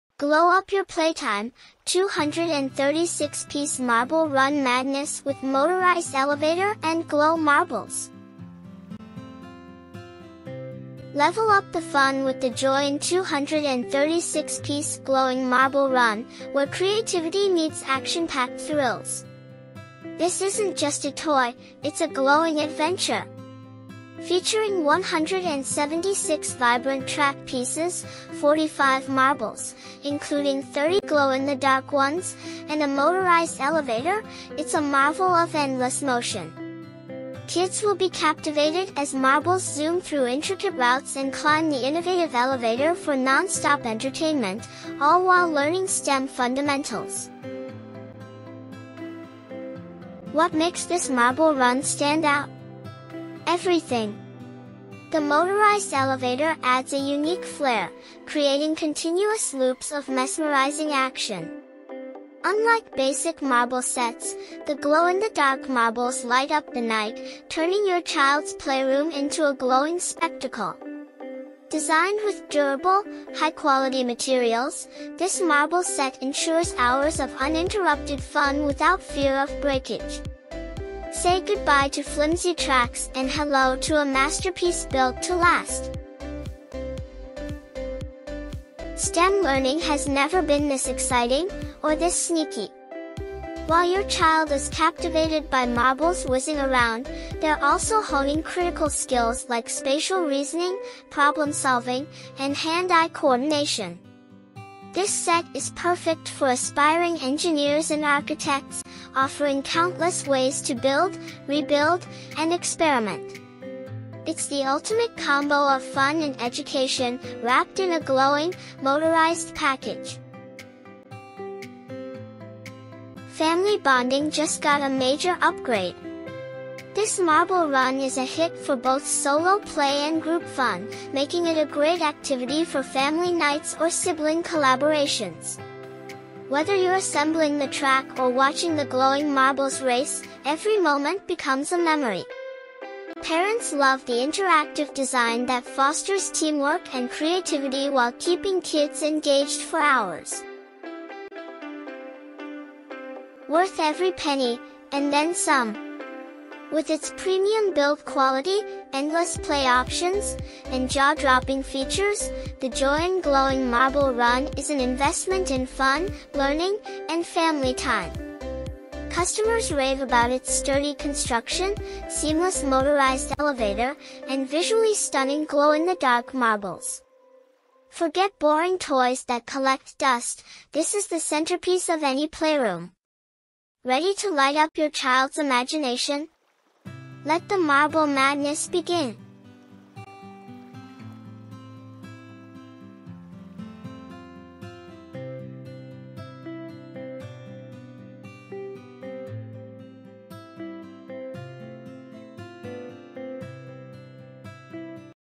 Glow Up Your Playtime: 236-Piece Marble Run Madness with Motorized Elevator & Glow Marbles!
Level up the fun with the JOYIN 236-Piece Glowing Marble Run, where creativity meets action-packed thrills. This isn’t just a toy; it’s a glowing adventure! Featuring 176 vibrant track pieces, 45 marbles (including 30 glow-in-the-dark ones), and a motorized elevator, it’s a marvel of endless motion. Kids will be captivated as marbles zoom through intricate routes and climb the innovative elevator for non-stop entertainment, all while learning STEM fundamentals.

What makes this marble run stand out? Everything. The motorized elevator adds a unique flair, creating continuous loops of mesmerizing action. Unlike basic marble sets, the glow-in-the-dark marbles light up the night, turning your child’s playroom into a glowing spectacle. Designed with durable, high-quality materials, this marble set ensures hours of uninterrupted fun without fear of breakage. Say goodbye to flimsy tracks and hello to a masterpiece built to last!

STEM learning has never been this exciting—or this sneaky. While your child is captivated by marbles whizzing around, they’re also honing critical skills like spatial reasoning, problem-solving, and hand-eye coordination. This set is perfect for aspiring engineers and architects, offering countless ways to build, rebuild, and experiment. It’s the ultimate combo of fun and education, wrapped in a glowing, motorized package.

Family bonding just got a major upgrade. This marble run is a hit for both solo play and group fun, making it a great activity for family nights or sibling collaborations. Whether you’re assembling the track or watching the glowing marbles race, every moment becomes a memory. Parents love the interactive design that fosters teamwork and creativity while keeping kids engaged for hours.

Worth every penny—and then some. With its premium build quality, endless play options, and jaw-dropping features, the JOYIN Glowing Marble Run is an investment in fun, learning, and family time. Customers rave about its sturdy construction, seamless motorized elevator, and visually stunning glow-in-the-dark marbles. Forget boring toys that collect dust—this is the centerpiece of any playroom. Ready to light up your child’s imagination? Let the marble madness begin!